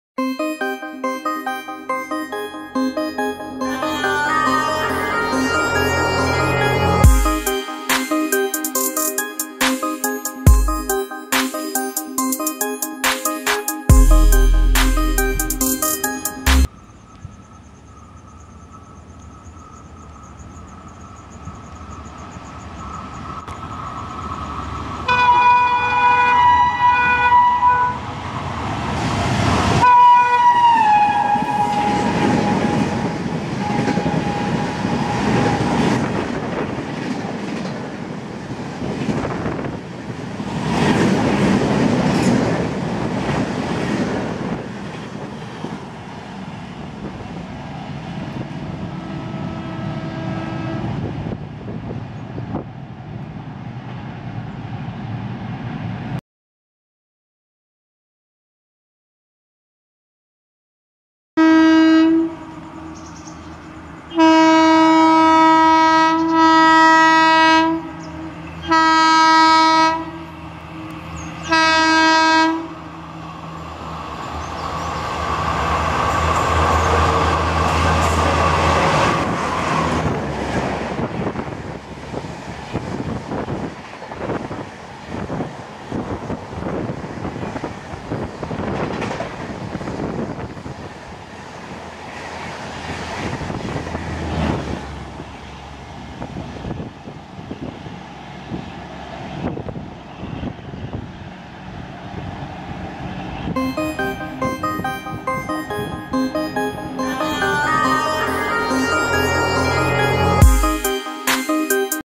High speed LHB trains- WAP7 Antyodaya vs WAP4 Humsafar
A high sped crossing between two LHB trains of South Eastern Railways-
On a humid monsoon afternoon TATA WAP7 22878 Ernakulam Howrah Antyodaya Express crosses SRC WAP4 powered 12504 Agartala Bengaluru Cantt Humsafar Express.